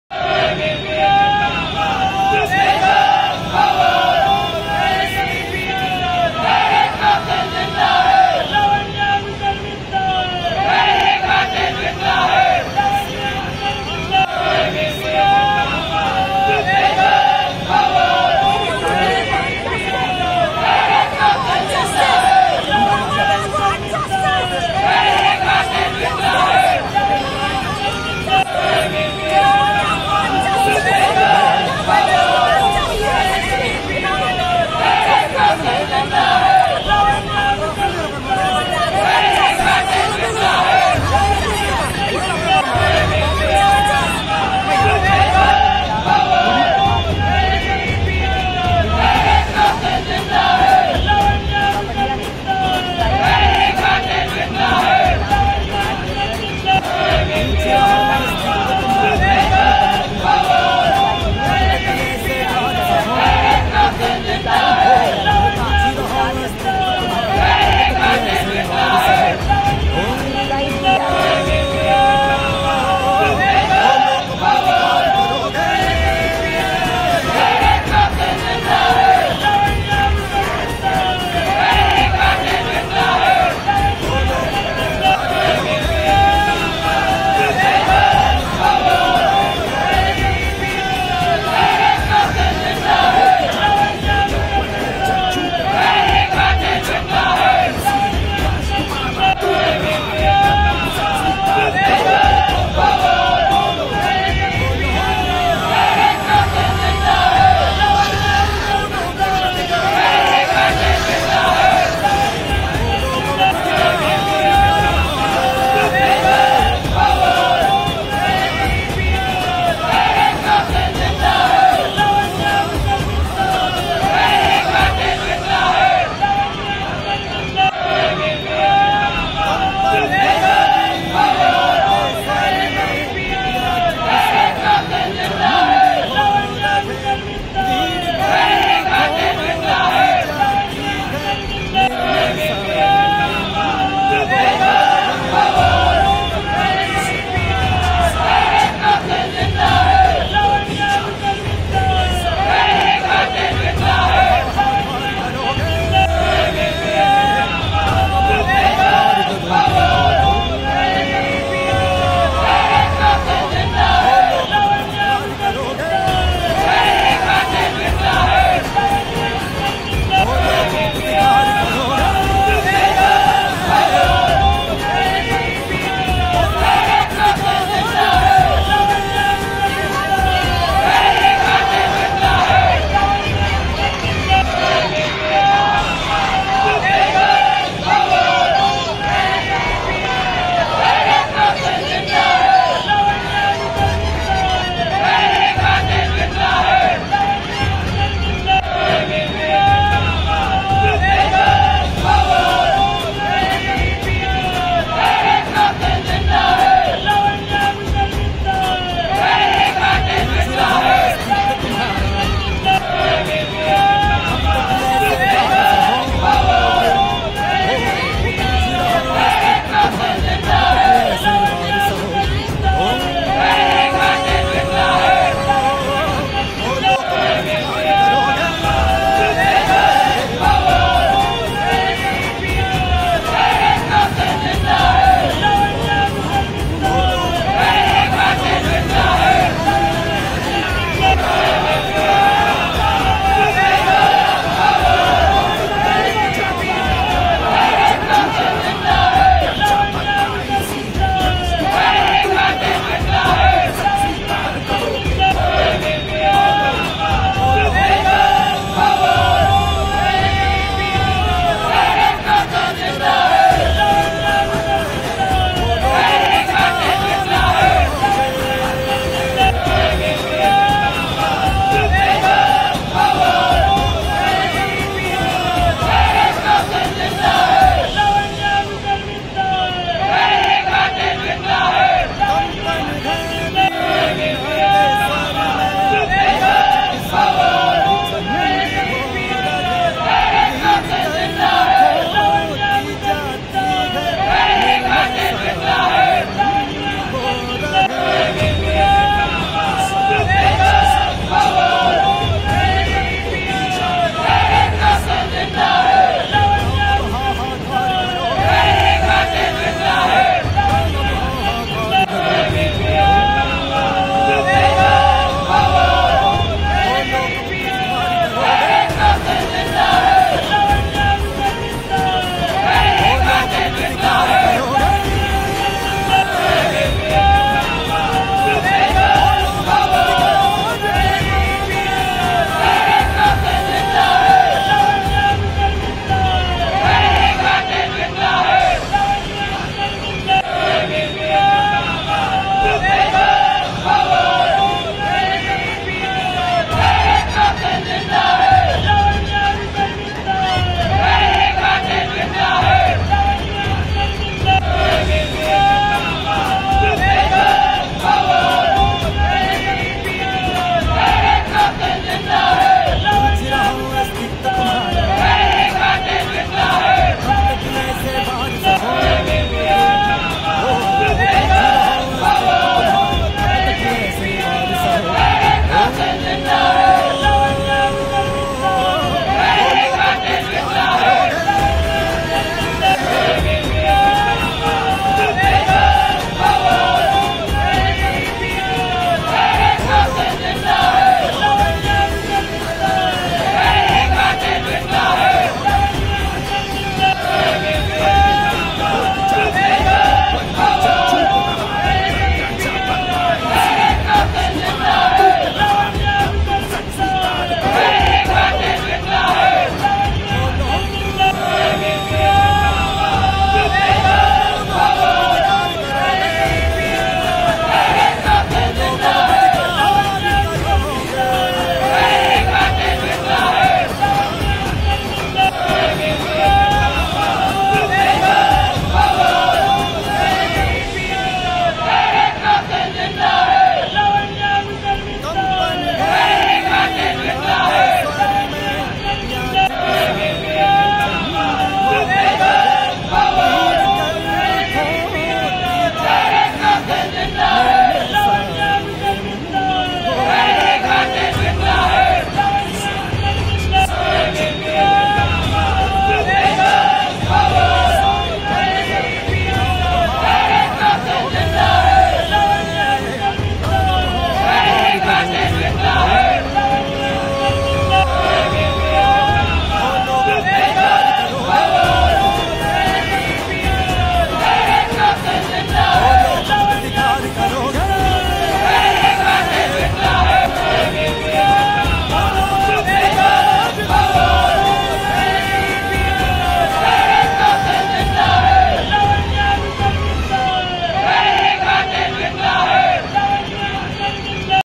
நாடு முழுக்க தொடரும் போராட்டம் || JusticeForLavanaya FightForLavanya || VSKDTN
லாவண்யா வுக்கு நீதி கேட்டு தொடர்ந்து நாடு முழுவதும் பல்வேறு இடங்களில் பல்வேறு விதமான போராட்டங்களை ABVP முன்னெடுத்து வருகிறது